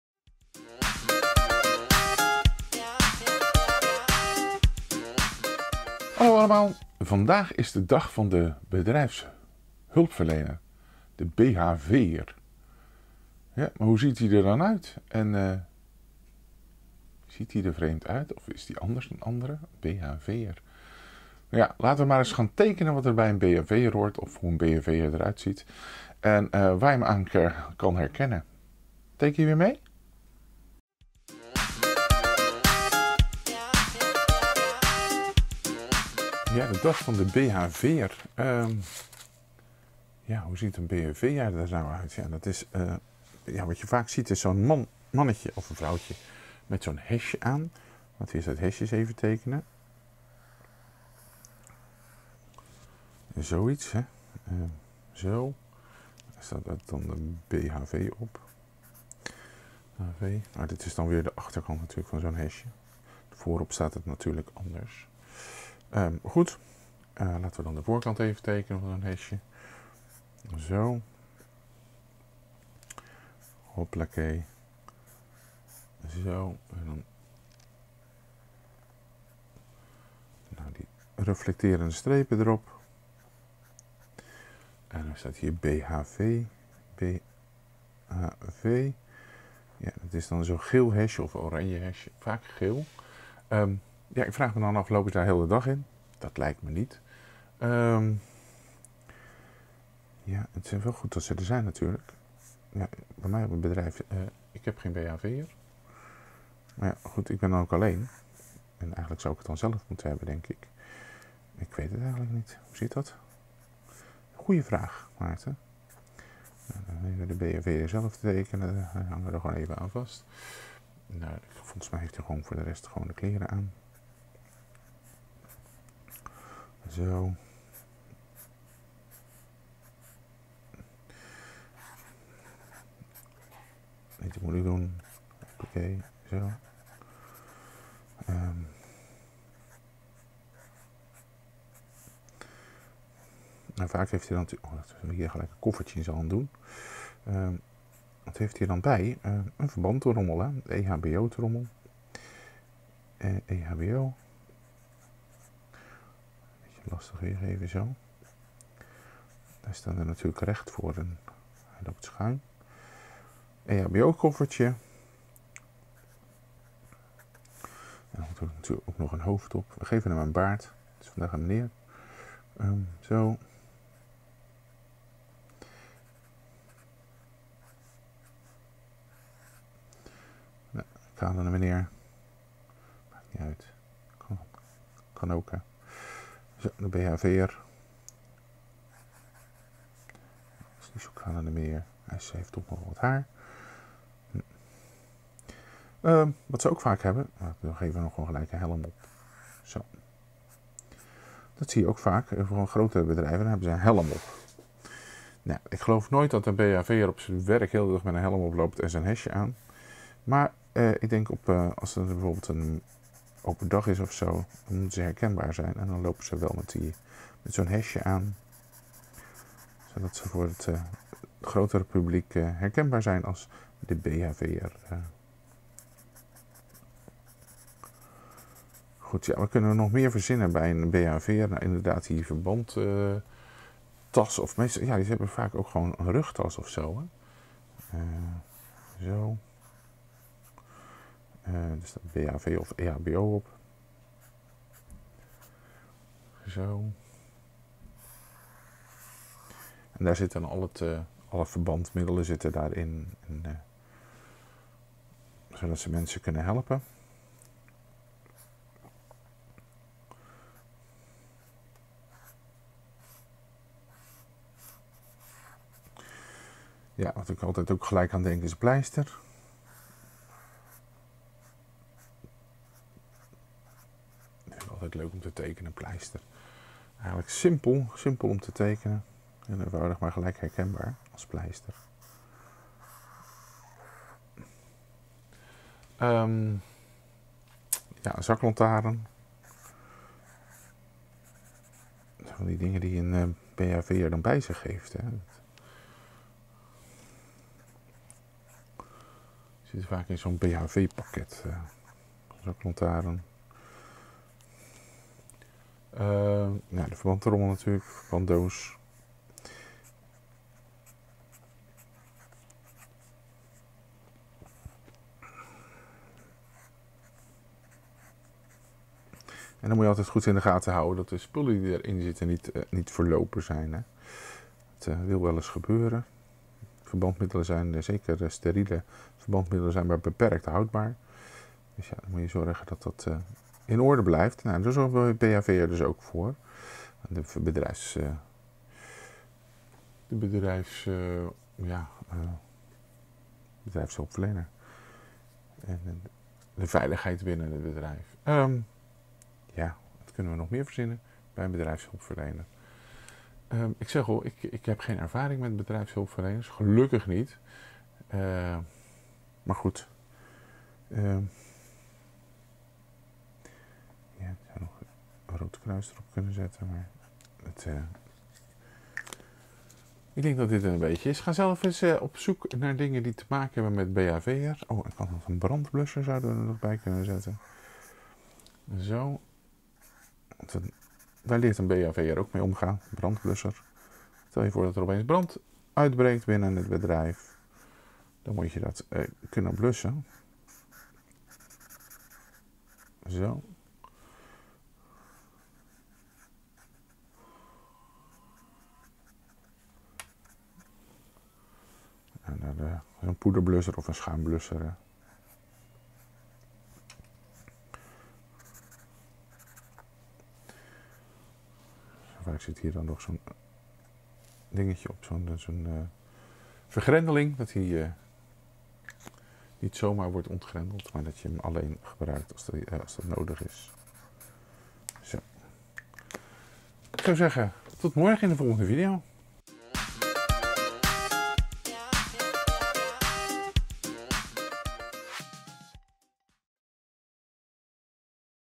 Hoe teken ik een bedrijfshulpverlener BHV
Vandaag is het de dag van de bedrijfs hulp verlener of de BHVer.
Laten we er eens een tekenen